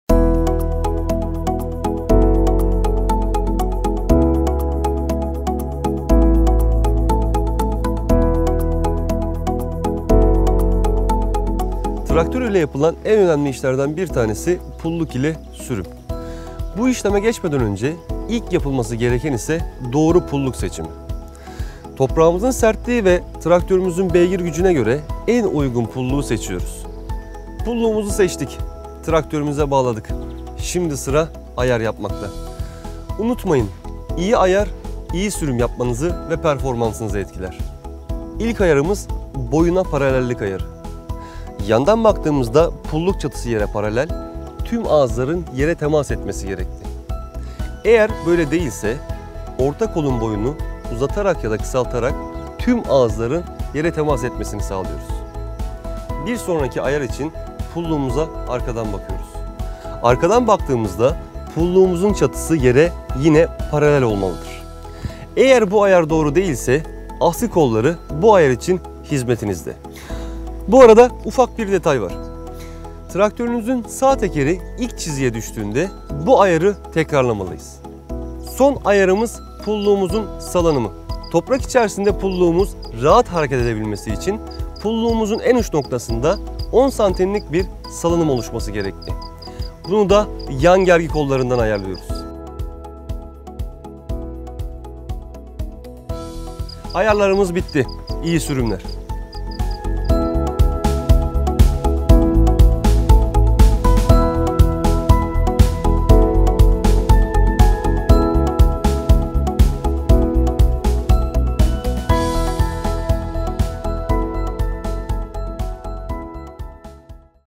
Pulluk Ayarları
Traktörünüze bağladığınız pulluğunuz ile sürüme girmeden önce yapacağınız ayarlar bu video ile anlatılmıştır.